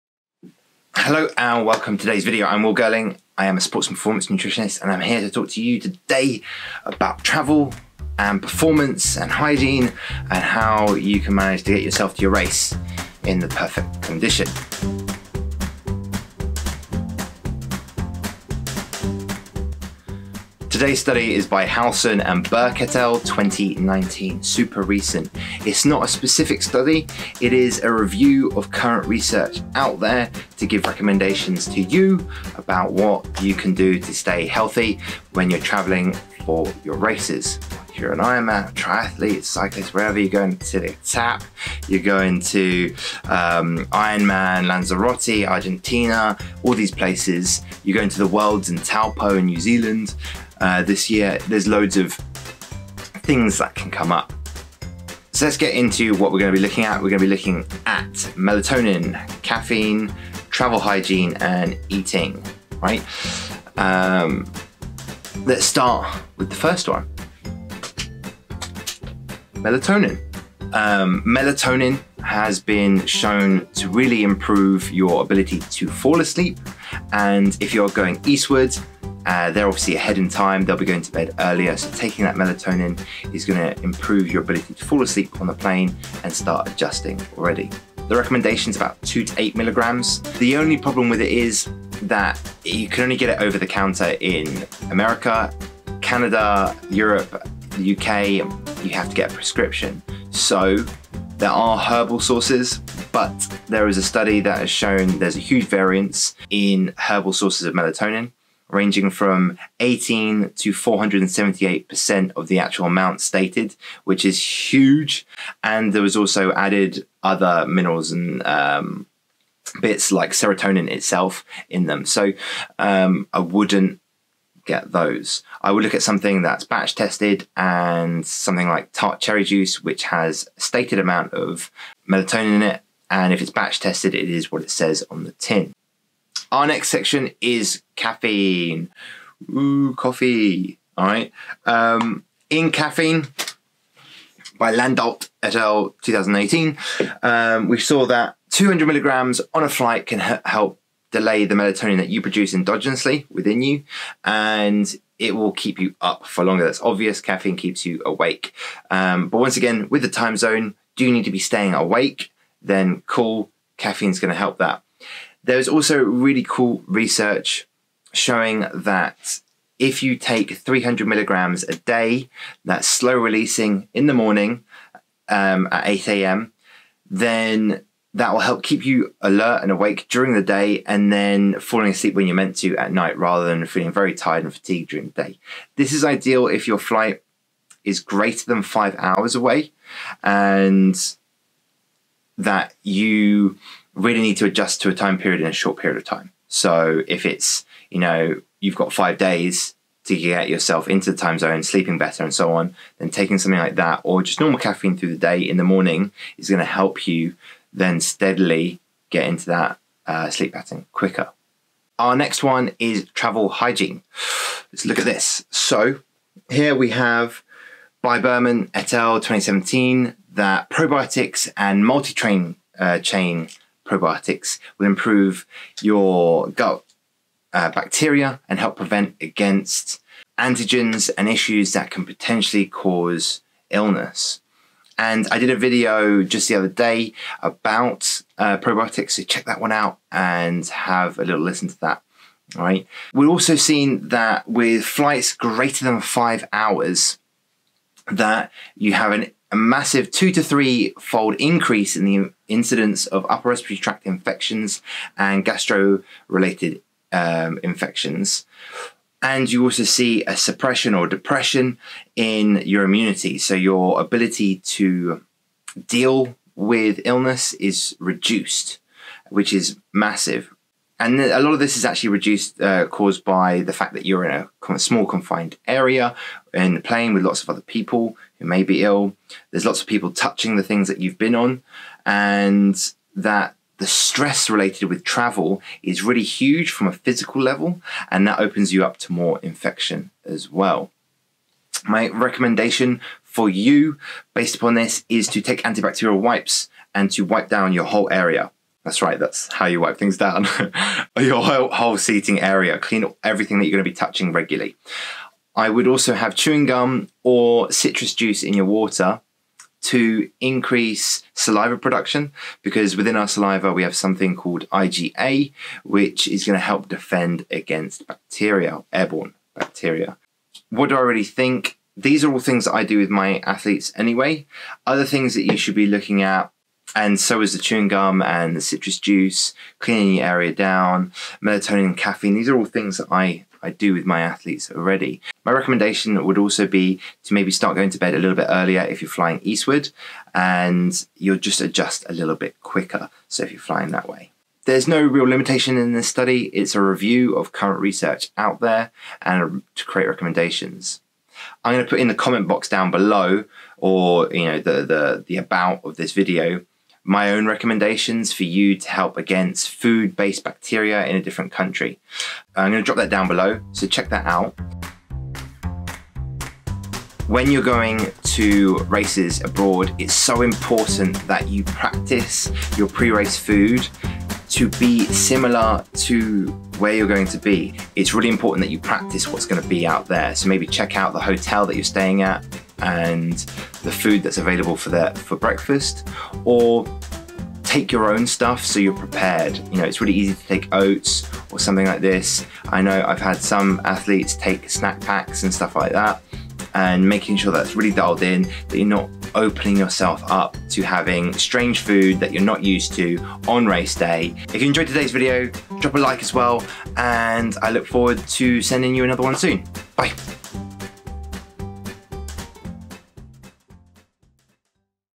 Race Day Travel - Nutrition and how to do it right!
My top tips while away to help you with you.
1. Practice good personal hygiene—keeping hands away from the face, undertaking frequent hand washing with soap or hand sanitizers, and keeping water bottles and other personal equipment clean
2. Undertake research on travel hygiene issues that is destination specific and prepare a targeted plan for the trip
3. Drink safe potable water. Use sealed bottled water if there are any doubts and do not use ice unless from a safe source
4. Use local knowledge to identify best restaurant and catering locations
5. When possible, check food storage facilities and practices of catering venues (check for general cleanliness, look for overcrowding of storage, mixing of raw and cooked food on the same shelves, overflowing containers)
6. Take extra care with buffet-style catering. Request freshly prepared dishes rather than preprepared and reheated meals (including takeaways). Note practices around duration and temperature of food holding for both hot and cold foods. Be aware of cross-contamination with utensils and cutting boards/serving plates
7. Avoid local street food due to lack of knowledge about their food hygiene practices
8. If undertaking self-catering, take care with food handling and storage
9. In countries with different standards around food/water hygiene, take care (or avoid) high-risk foods
• Nonpotable water from the tap, including ice made from this water or foods that have been washed in it
• undercooked and raw meat, shellfish, seafood (including sushi)
• raw eggs
• unpasteurized dairy foods
• salads and raw vegetables
• fruit that cannot be peeled
• food that has been kept warm for an indeterminate amount of time
confronted